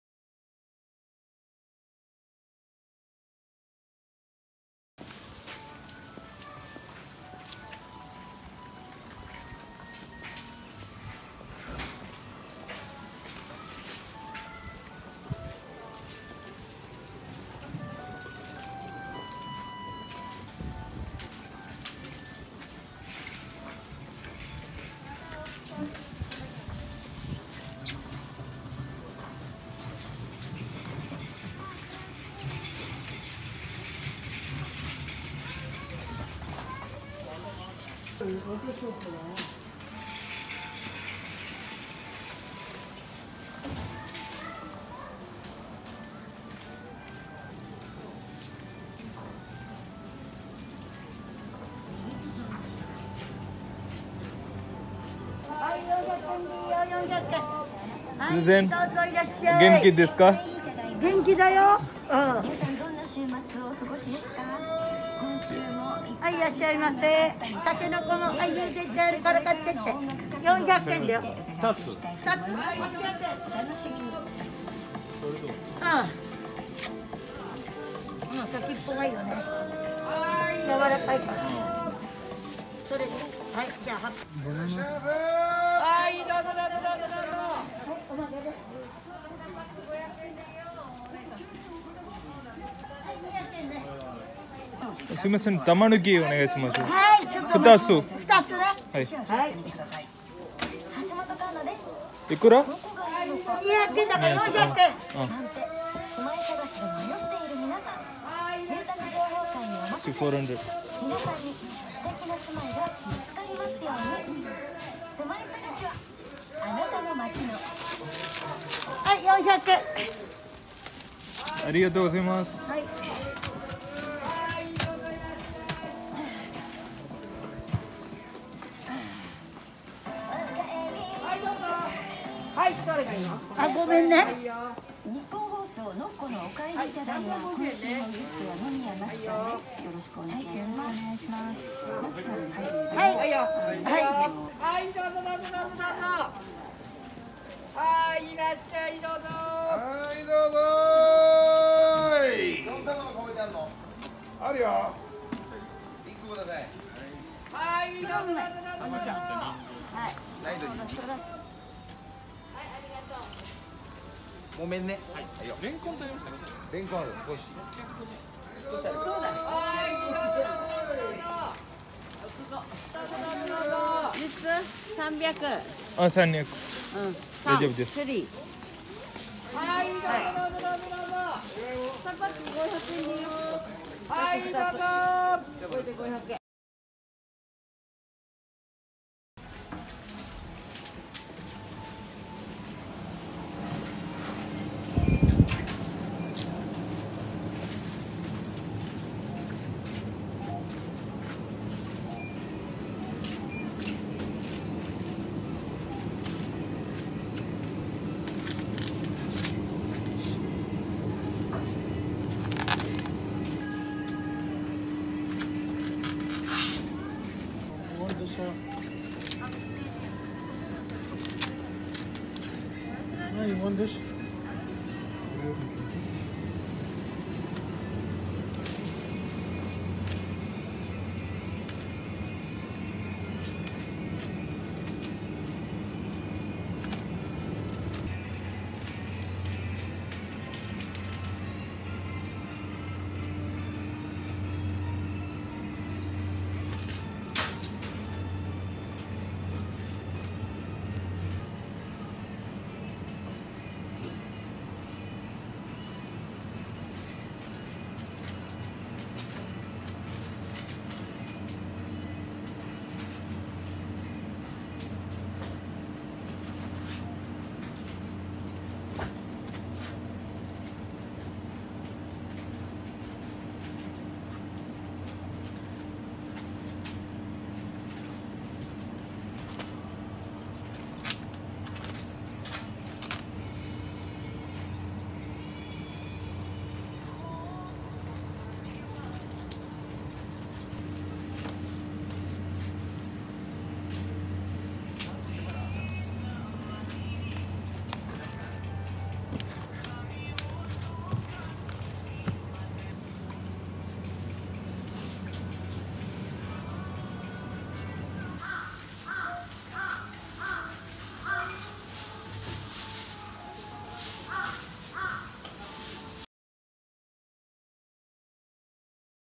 Ojima Market - A walkthrough
Ojima is a place in Koto-ku ward within Tokyo. This video covers the small market in Ojima area. Good place for weekend vegetable purchase